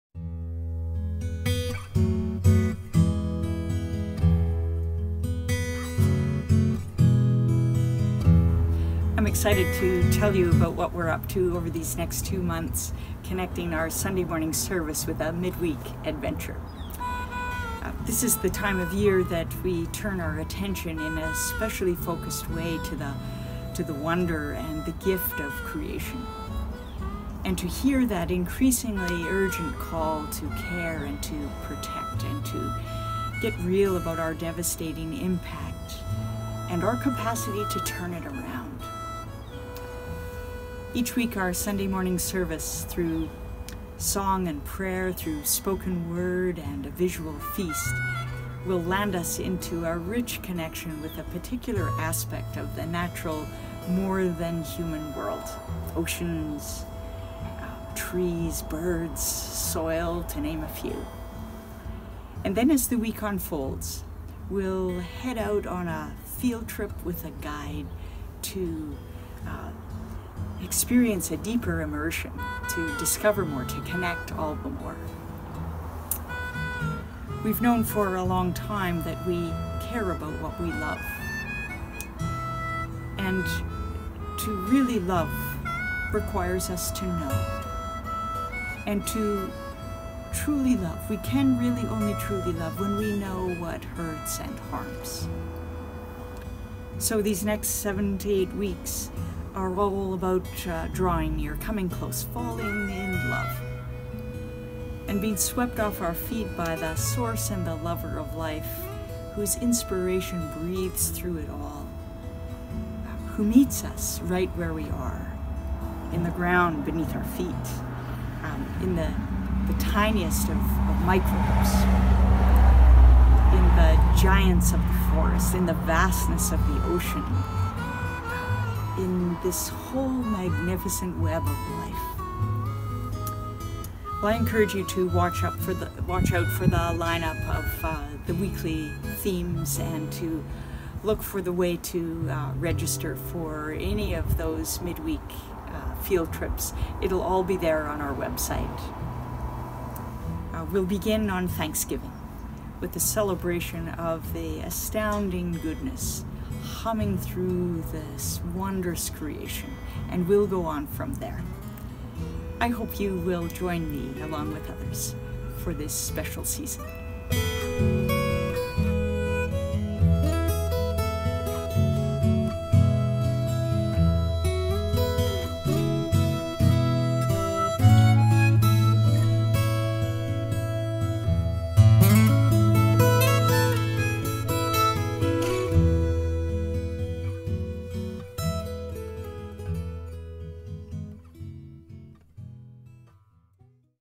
the wonder and gift of creation
From Thanksgiving through to late November we’re venturing something new.

Each week we’ll be pairing the experience of our Sunday morning worship with a mid-week field trip providing for a deeper immersion in the theme as we celebrate the wonder of God’s creation.